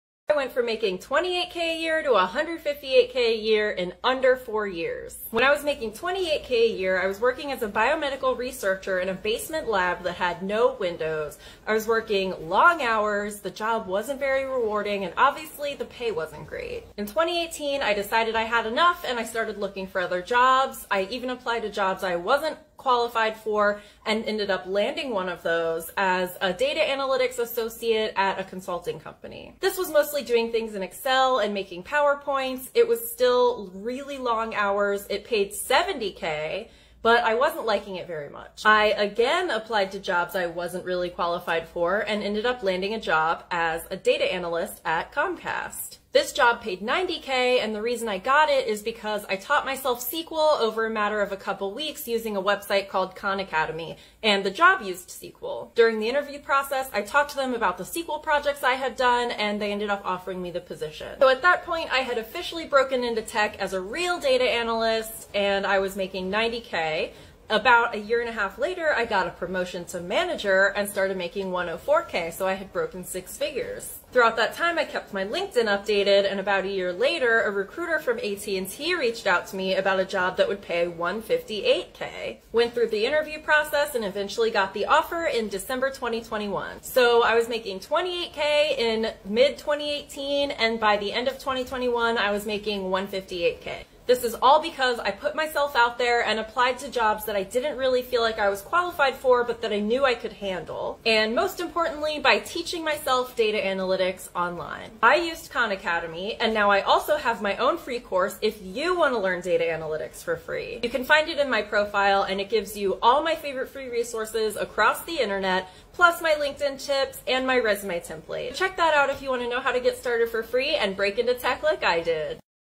How to Break Into tech with Data Analytics and Get a Six Figure Salary!
This is what worked for me:
1. Teach yourself the in-demand skills.
2. Apply for jobs even if you think you're not qualified. If it's marked as entry level, APPLY! (Even if you don't meet the "requirements").
3. Continue to hone your skills while on the job (while getting paid!) to get promotions and more job offers.